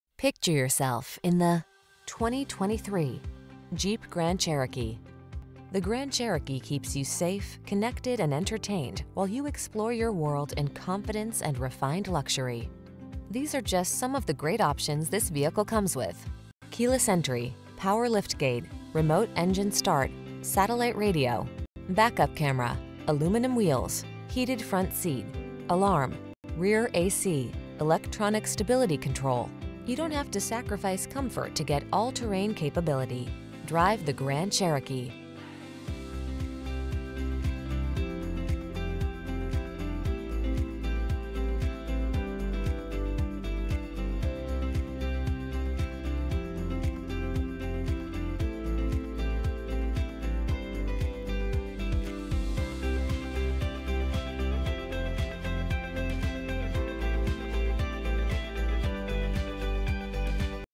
2023 Jeep Grand Cherokee L at Oxmoor CDJR | Louisville & Lexington, KY C14046
Diamond Black Crystal Pearlcoat New 2023 Jeep Grand Cherokee L Laredo available at Oxmoor CDJR in Louisville, Kentucky. Servicing the Lexington, Elizabethtown, KY New Albany, IN Jeffersonville, IN area.

Oxmoor CDJR
4520 Shelbyville Rd